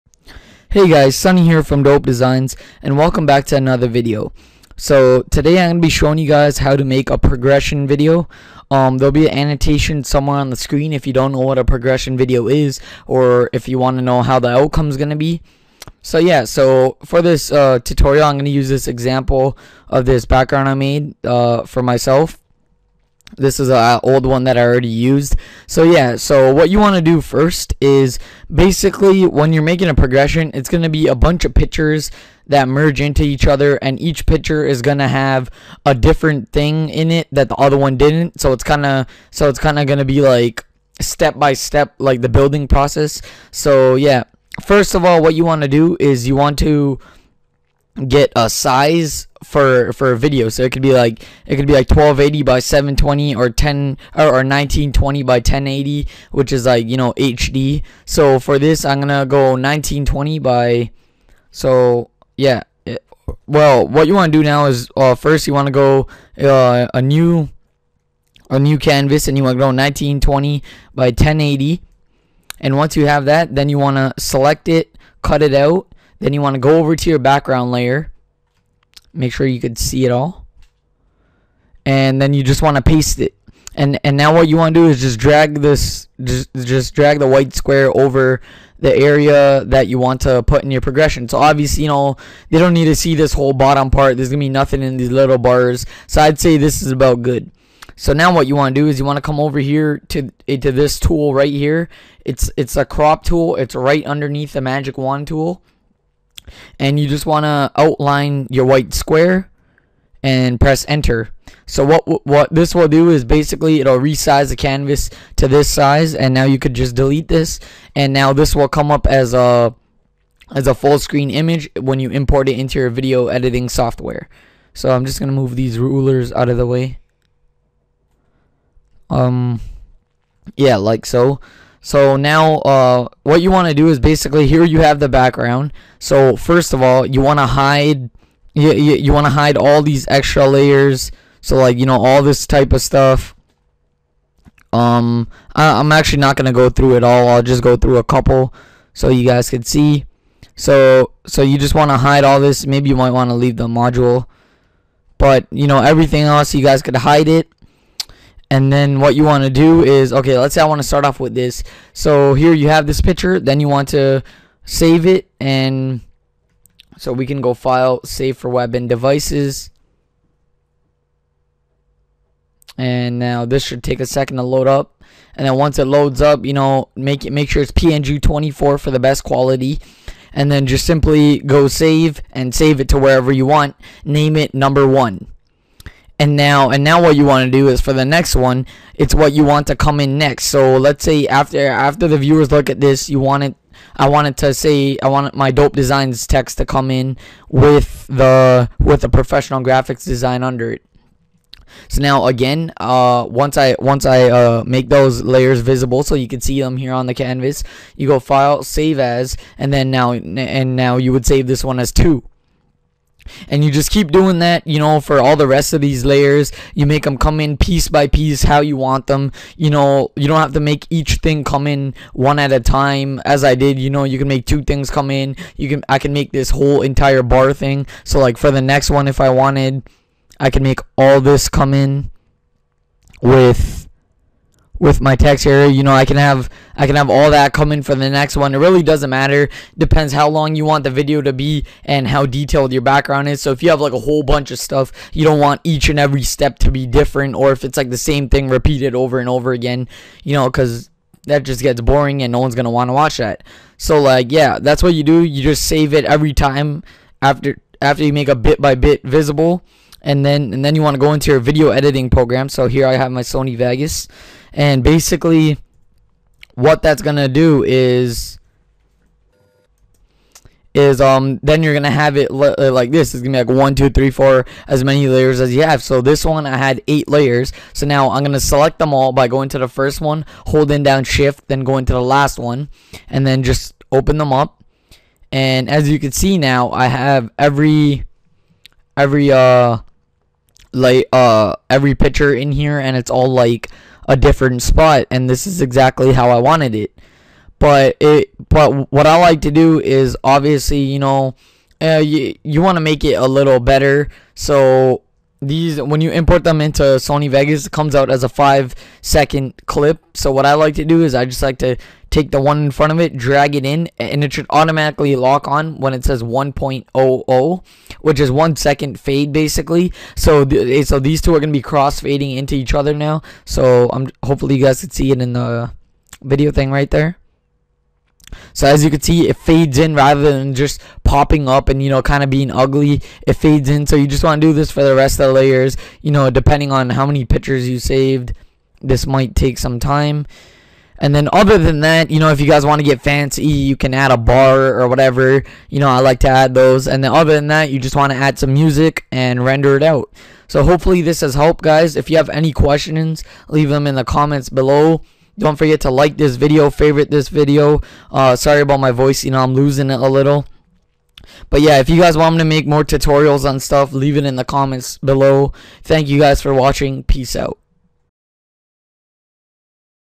How To Make A Video Progression (GFX SHOWCASE TUTORIAL) | By Dope Designs
if you have any questions or tutorial requests leave them in the comments below!

needed: photoshop, vidoe editing software (sonyevegas)

HELPS ME ALOT!